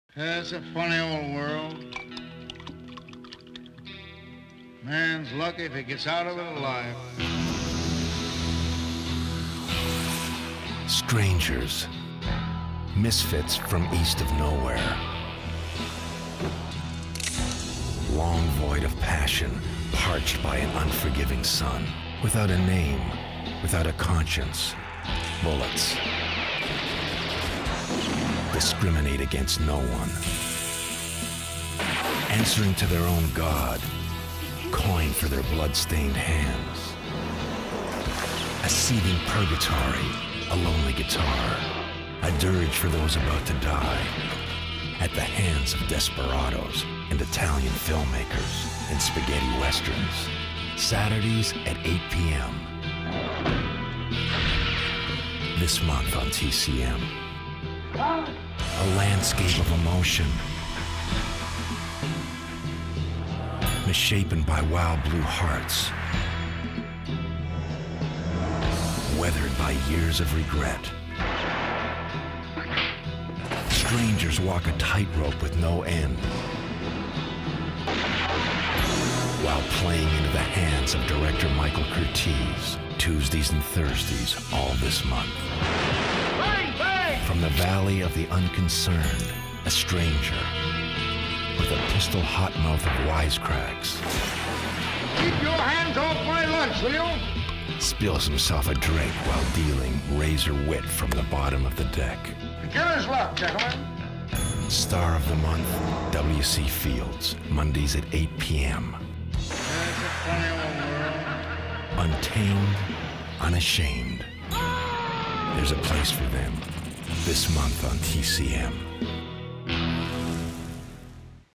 TCM This Month June: Spaghetti Westerns/W.C.Fields/Michael Curtiz
The challenge: combine W.C. Fields, Spaghetti Westerns, and the films of Michael Curtiz in a cohesive and meaningful way. "I had a blast cutting these 2 minute and 1 minute promos for TCM with writer/producers Kelli Christiansen and Scott McGee many years ago" says editor Michael Koepenick. "The music and the VO by Robbie Robertson really help to create the perfect mood for murder, mayhem and a few good laughs". Ben Prisk did a great job providing the graphics.